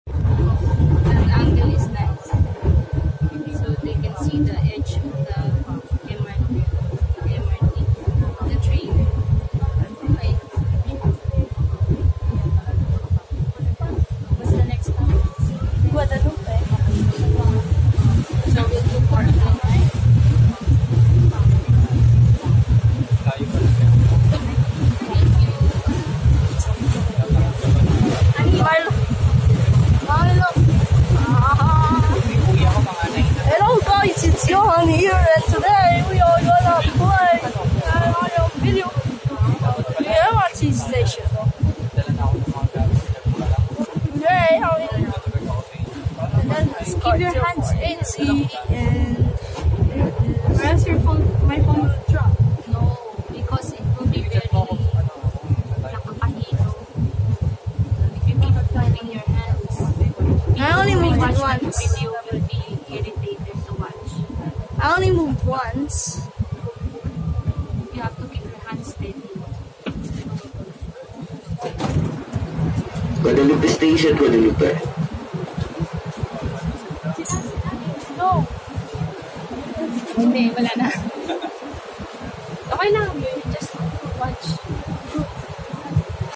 MRT Round Trip Part 2
Kuya wasn't Keeping his hand steady and he only moved ones and We're Going to Allight at Magallanes Station